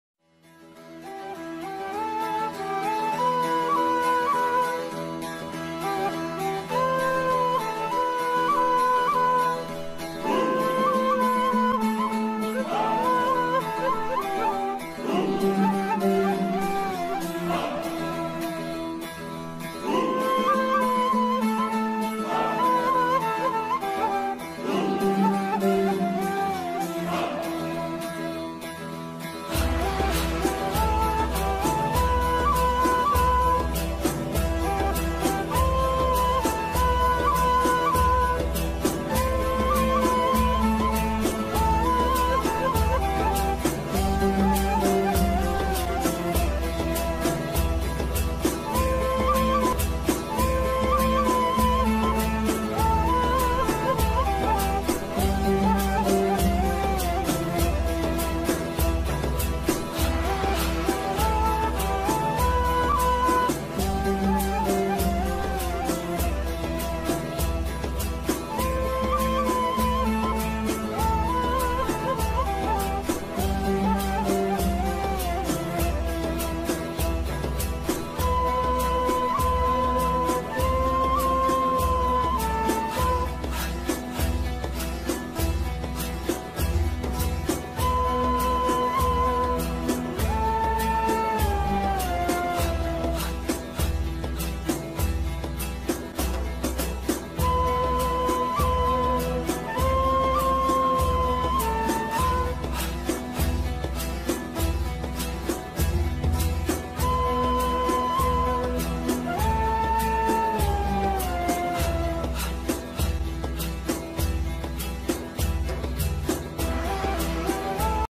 Engin Altan (Ertugrul Ghazi) in Pakistan - Ertugrul visit to Lahore | Wowrena TV
Recently Engin Altan aka Ertugrul visits Pakistan to sign a contract with an industry. Ertugrul's visit to Lahore was appreciated by the Pakistani people.